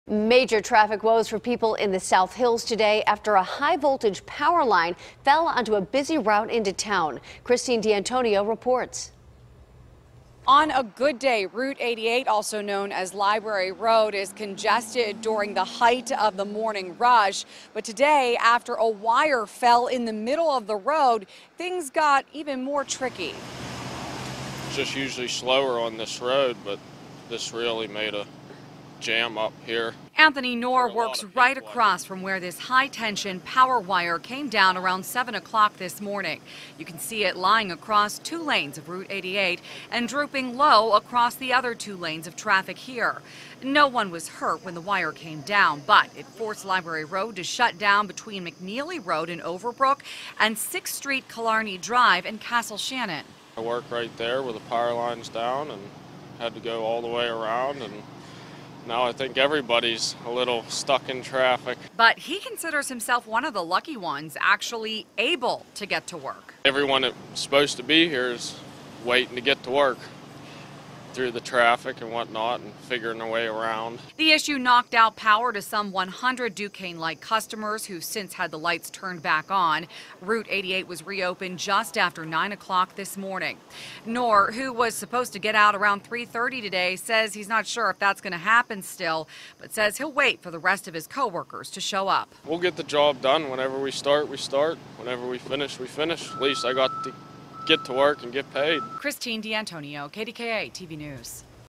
Downed Power Line Closes Library Road In Overbrook
A downed power line had a big impact on the morning commute.